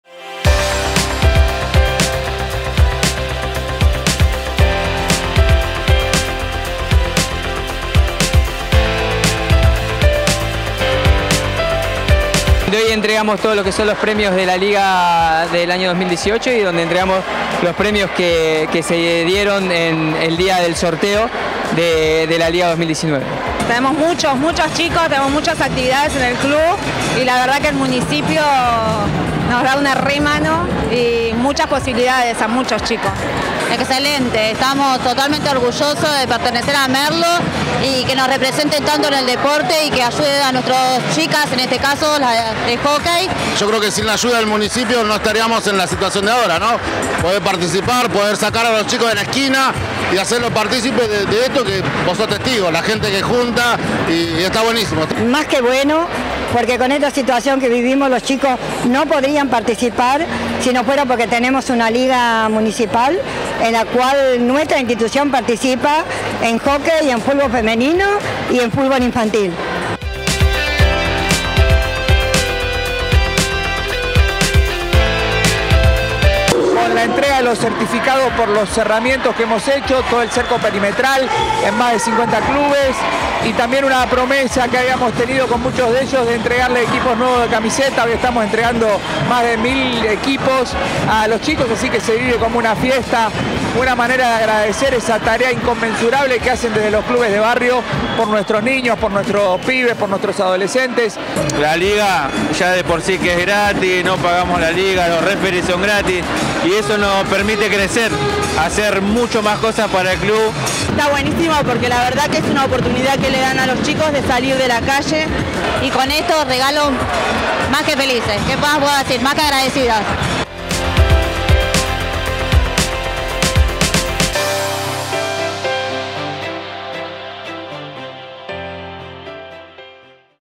Las Ligas Municipales recibieron equipos deportivos
Se hizo entrega de más de mil elementos e indumentaria a las instituciones barriales que forman parte de las ligas municipales de Fútbol y Hockey.